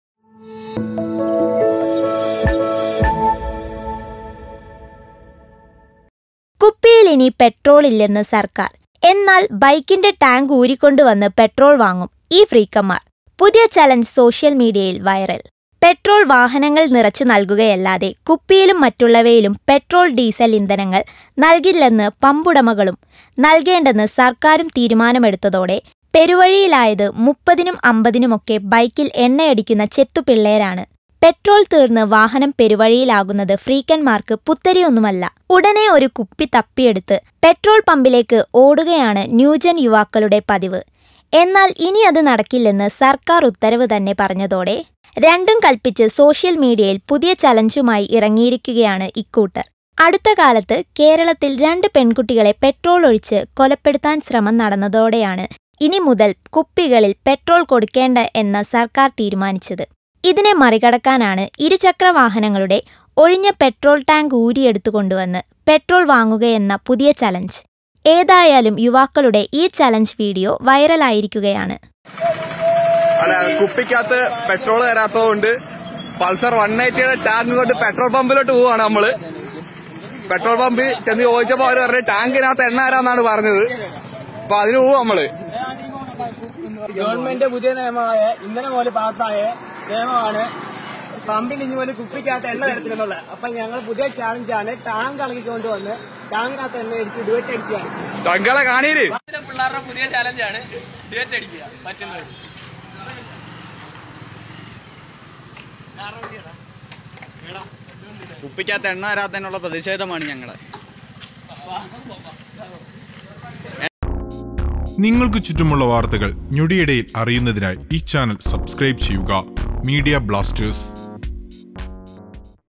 കുപ്പിയില്‍ ഇനി പെട്രോളില്ലെന്ന് സര്‍ക്കാര്‍ പെട്രോൾ ടാങ്ക് അഴിച്ച് കൊണ്ട് വന്ന് ഈ ചുണക്കുട്ടികൾ
Watch കുപ്പിയില്‍ ഇനി പെട്രോളില്ലെന്ന് സര്‍ക്കാര്‍ ബൈക്കിൻ്റെ പെട്രോൾ ടാങ്ക് അഴിച്ച് കൊണ്ട് വന്ന് ഈ ചുണക്കുട്ടികൾ Funny Boy's Rocks